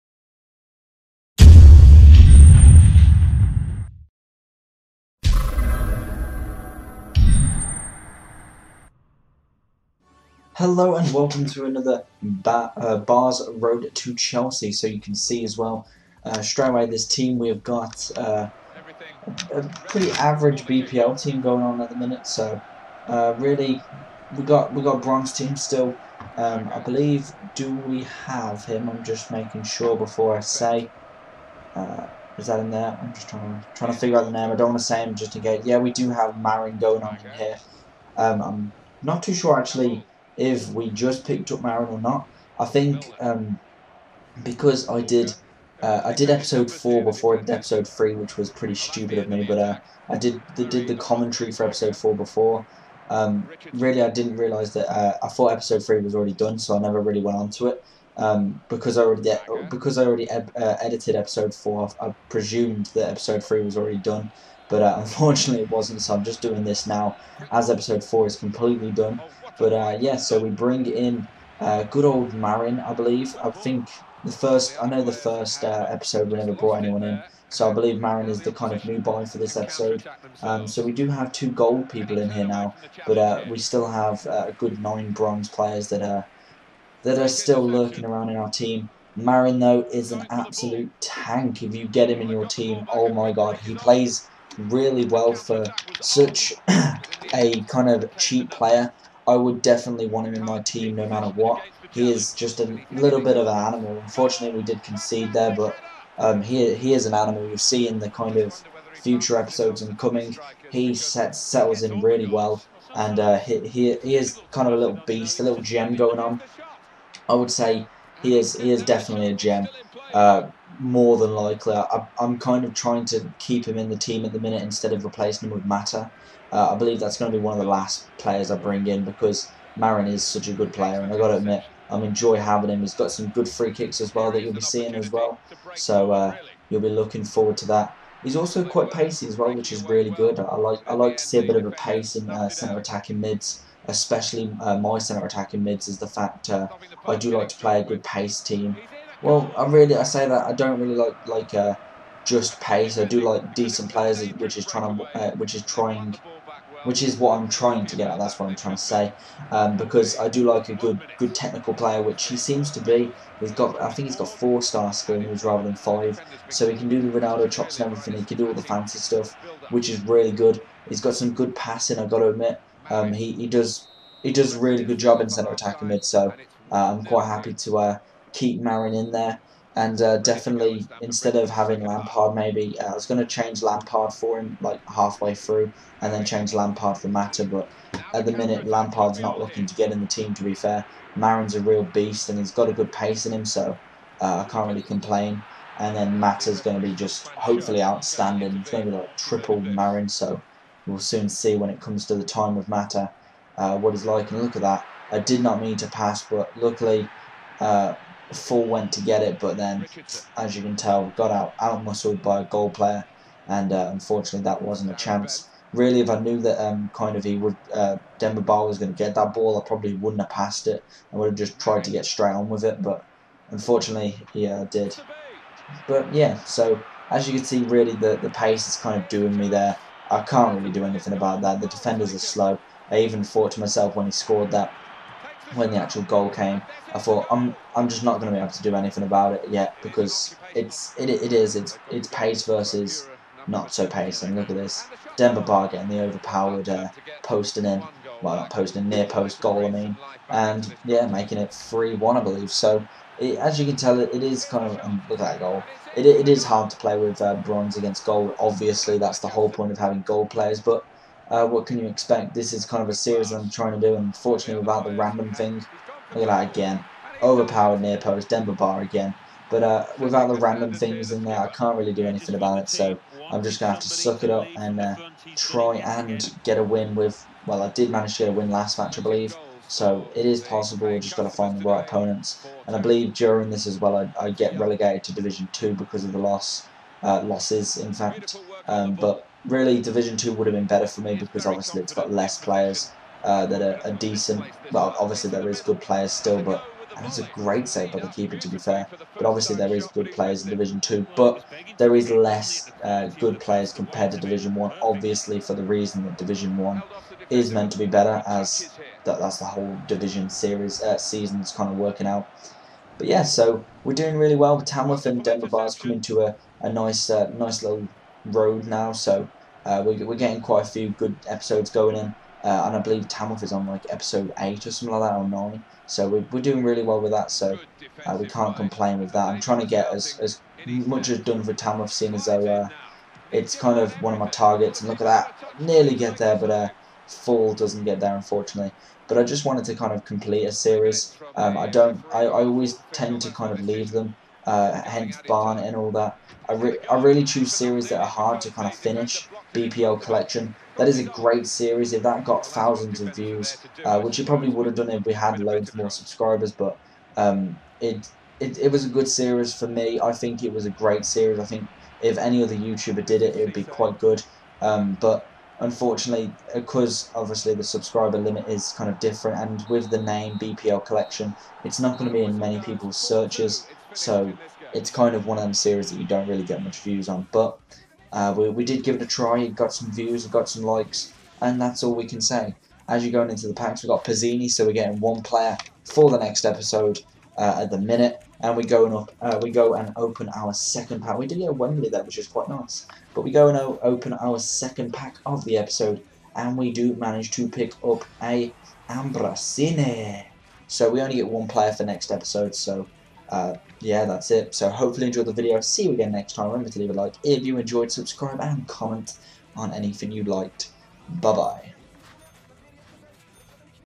Ba's Road To Chelsea - Episode 3
Welcome to episode 3 of the Dember Ba's Road To Chelsea. We bring in a new player in this episode so stick around to see him, amazing player if i do say so myself.

Also we have obviously the two new packs coming at the end of the episode to see if we get anyone for episode 4, hope you enjoy the episode and see you next time.
Check us out at:
Xbox:
NoXG x James
NoXG x Alex
NoXG x Cotton
NoXG x Ala